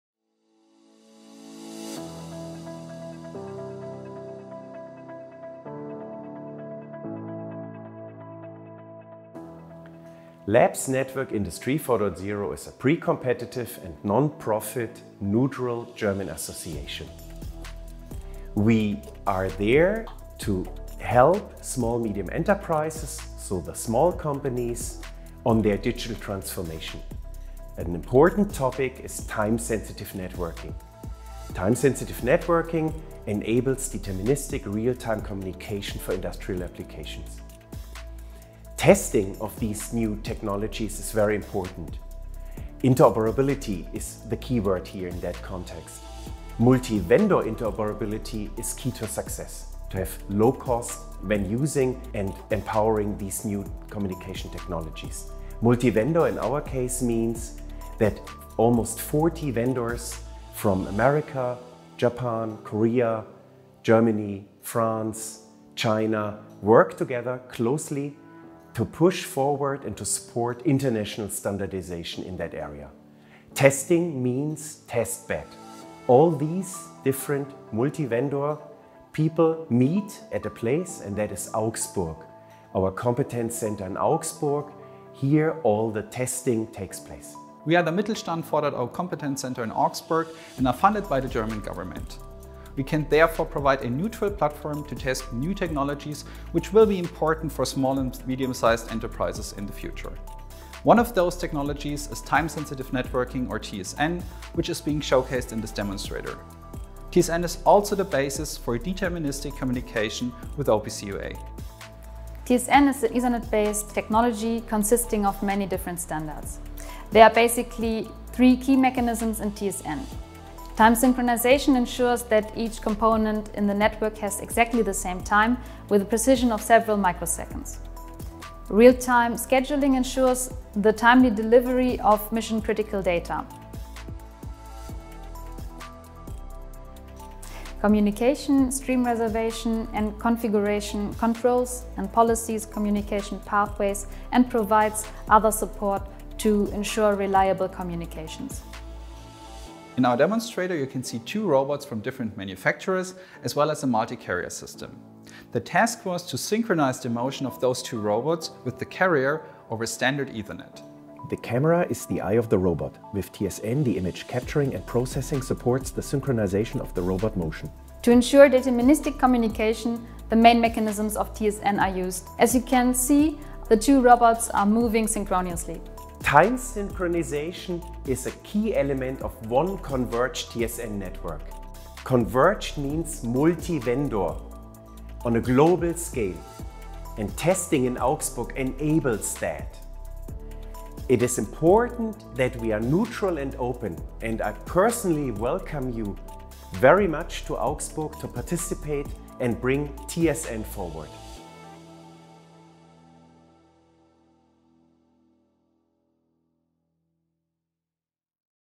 Testbed TSN (Time-Sensitive Networking)
We are an open, neutral and pre-competitive governmentally financed testbed hosted by the Kompetenzzentrum 4.0 Augsburg for the validation of extensions to the IEEE 802.1 standard family for time-sensitive networking (TSN) based on SME use cases.

The extensions make it possible to run heterogeneous real-time applications in a single TSN network for industrial communication solutions in multi-vendor realizations.

Among the 36 partners, there are 14 SME partners. Liaison contracts with the IEEE 802.1 Working Group and OPC Foundation exist. The demonstrator consists of two robots and a linear drive as well as an industrial camera with a multi-vendor realization.

The future of converged TSN networked operates all devices time synchronously.